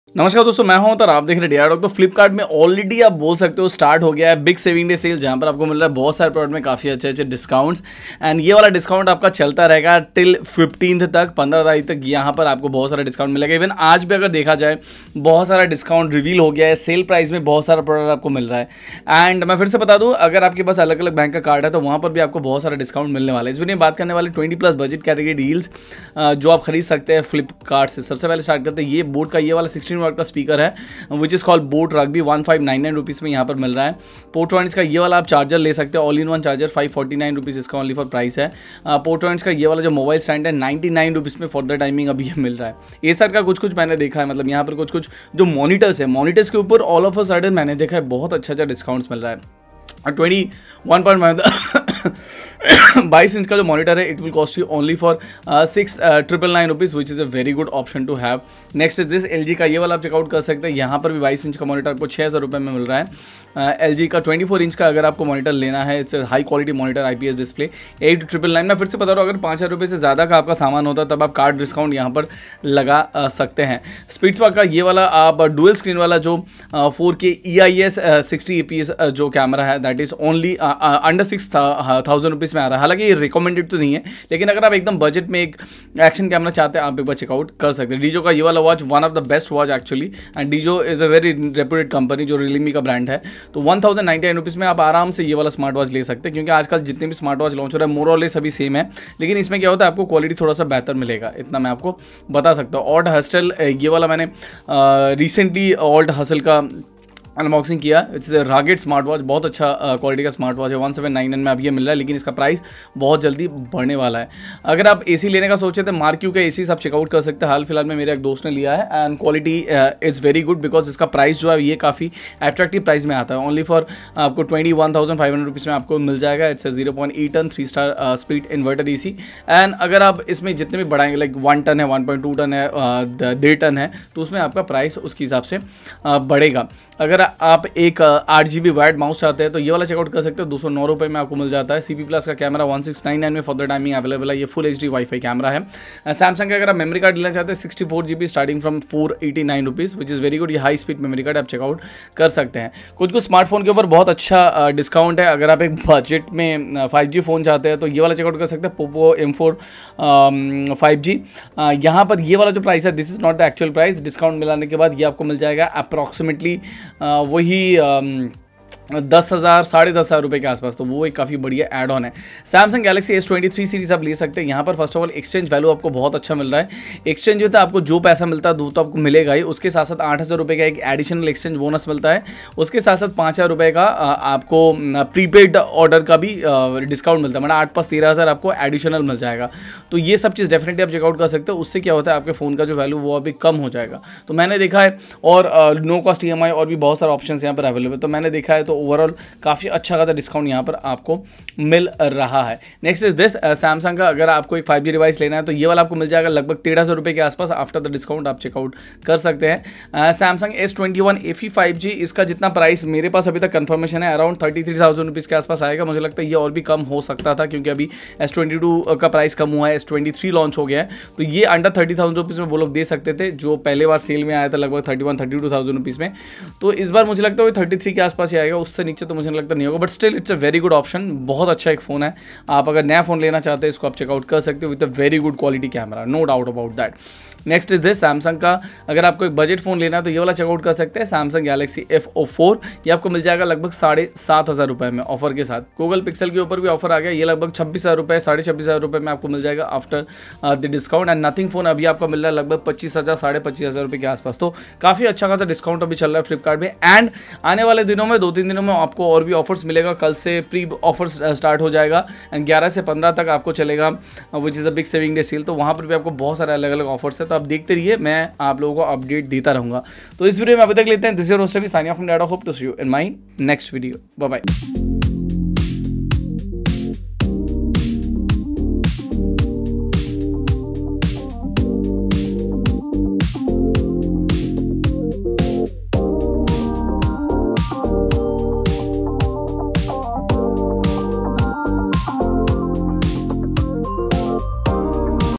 20+ Best Budget Deals on Flipkart BIG SAVING DAYS SALE March 2023
20+ Best Budget Deals  on Flipkart  BIG SAVING DAYS SALE March 2023
boAt Rugby Plus
Portronics 40 W
Portronics POR-122 MODESK
acer 21.5 inch Full HD
LG MK400 21.5 inch
LG 24 inch Full HD LED
Fitspark Eagle i Max
DIZO Watch 2 Sports
alt Hustle
MarQ by Flipkart 0.8 Ton Split Inverter AC
Arctic Fox Wired USB Gaming Mouse
CP PLUS E-24A FULL HD
SAMSUNG Evo Plus 64 GB
POCO M4 5G
SAMSUNG Galaxy S23 5G
SAMSUNG Galaxy F23 5G
SAMSUNG Galaxy S21 FE 5G
SAMSUNG Galaxy F04
Google Pixel 6a
Nothing Phone (1)
Let's be on Social :
Thanks,
Data Dock